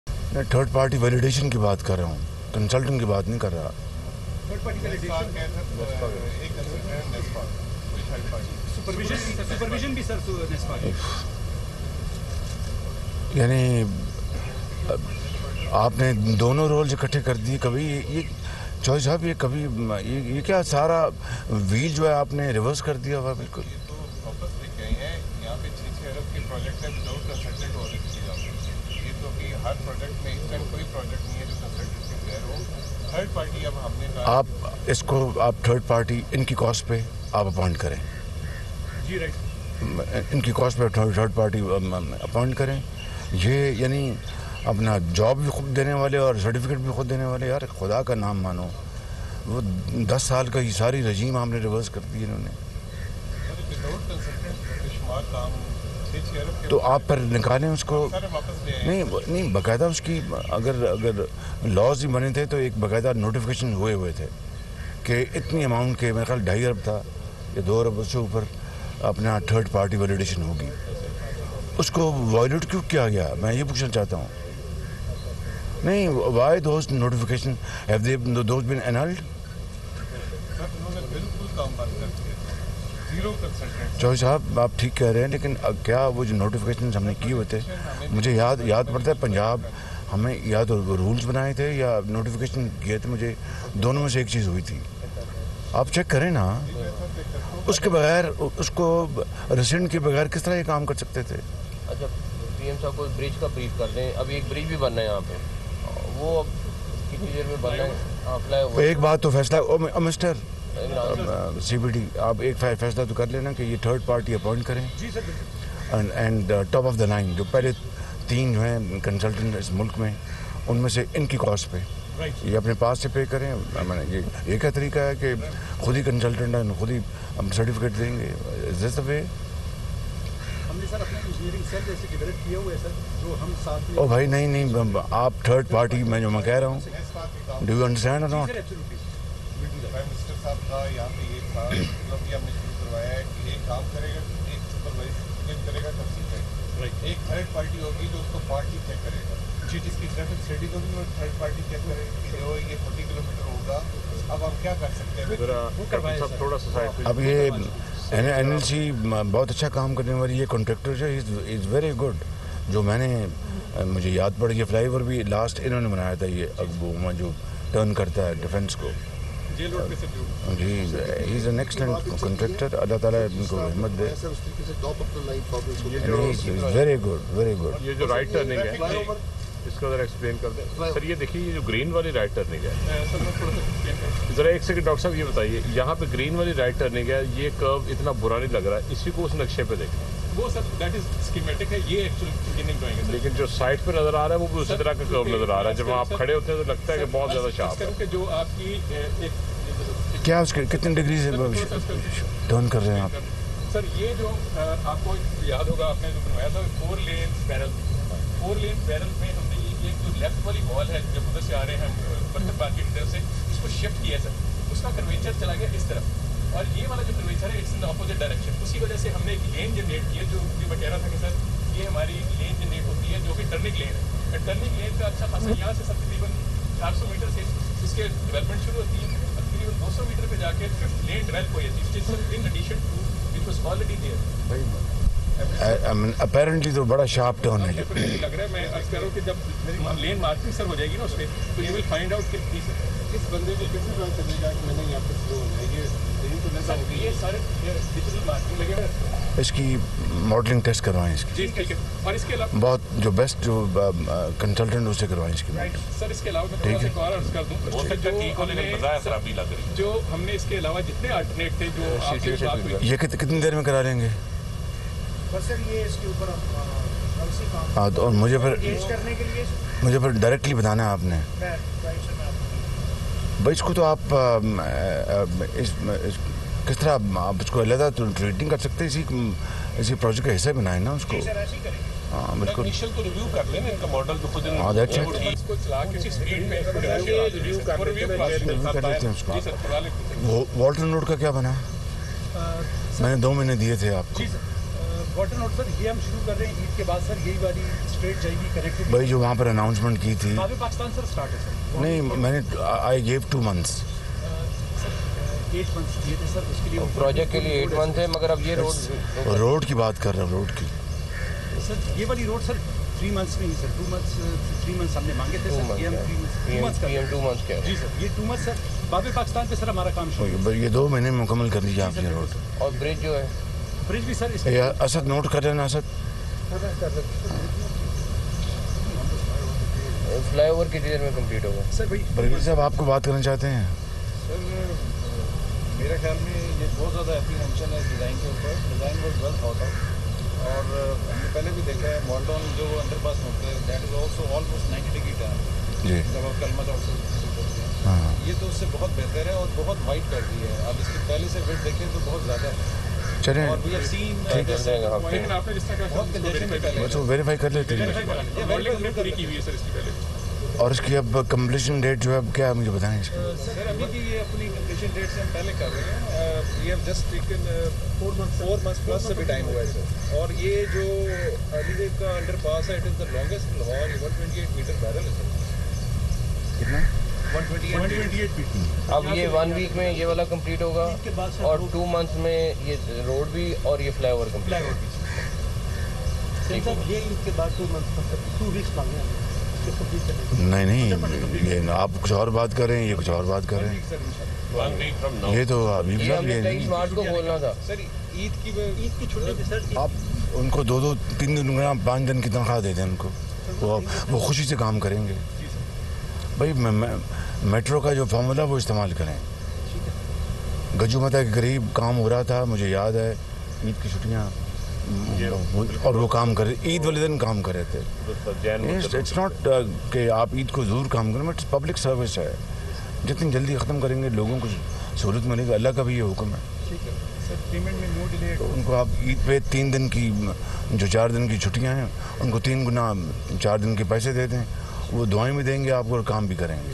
Jitni jaldi Kaam Karen Ge Logo Ko Shaulat Mile Gi : Shehbaz Sharif | Dunya News
Prime Minister Shehbaz Sharif and Care Taker CM Punjab Mohsin Naqvi Visit To Ongoing Development Projects In Lahore